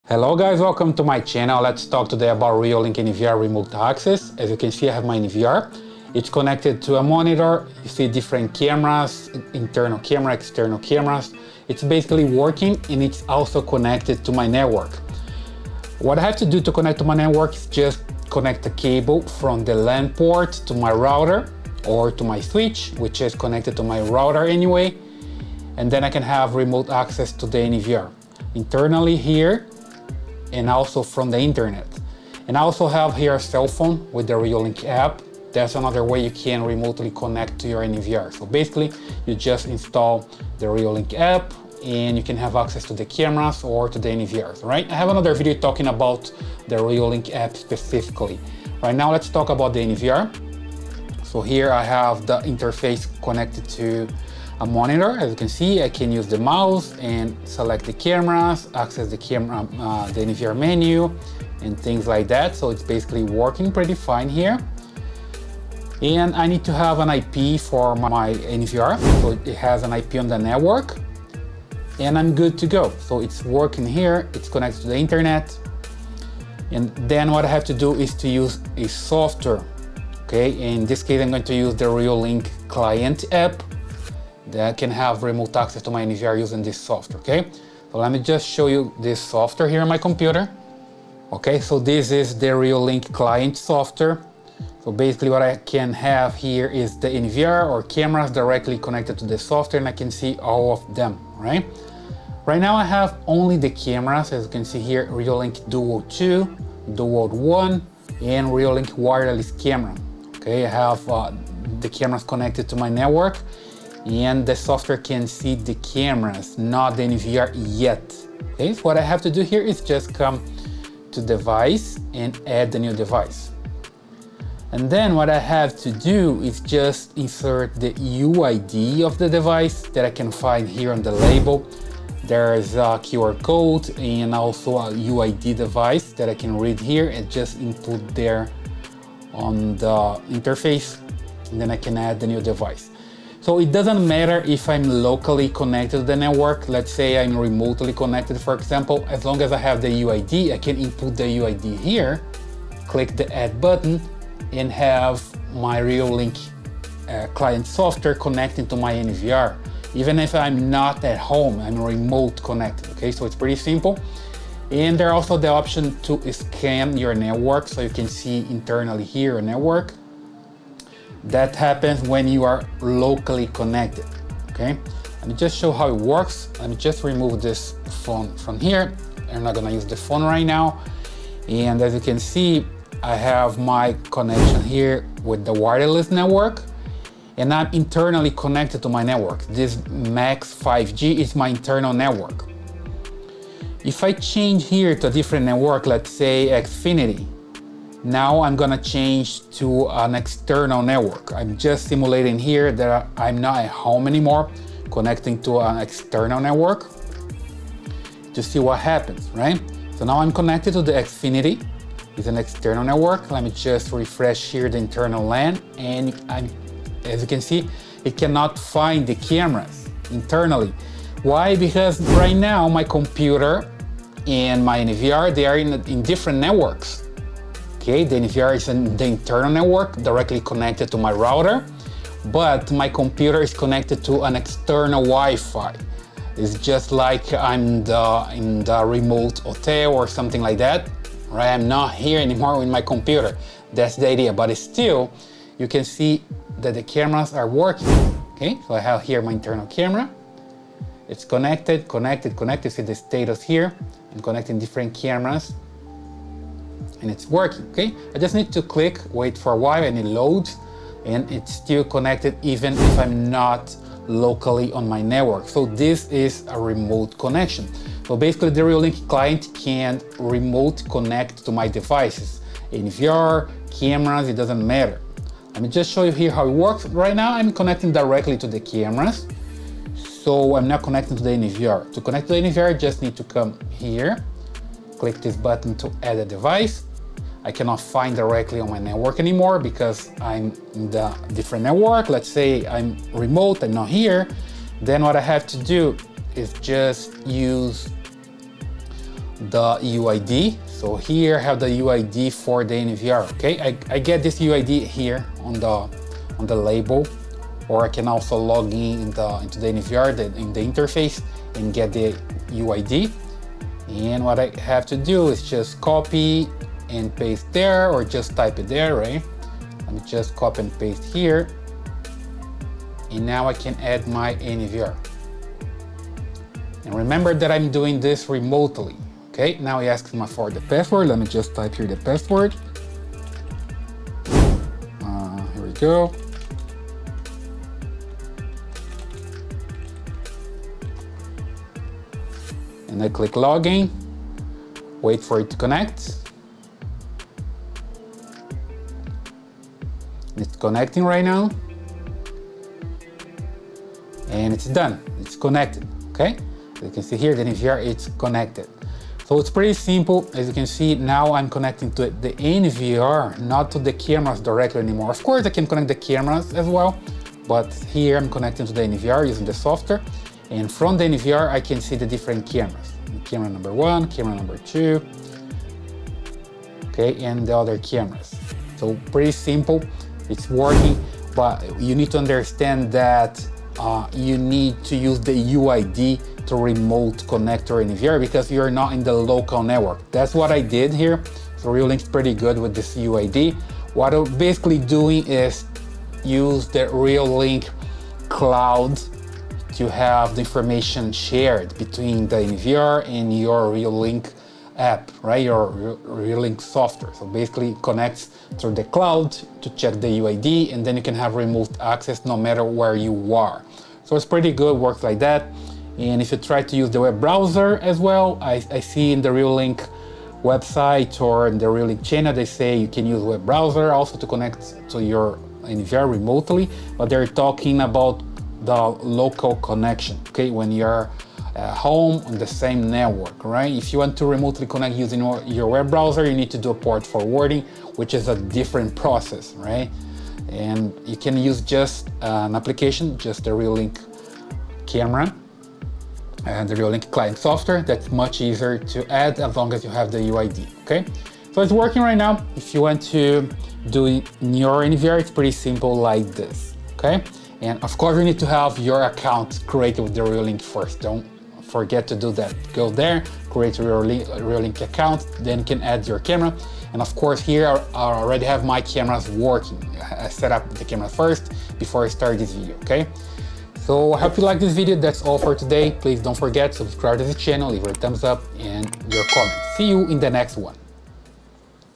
Reolink NVR Remote Access (via Reolink Client App)
In this video I explain how to have remote access to a Reolink NVR using the Reolink Client Software. It's necessary to have the cameras and recorder connected to a network and to the Internet.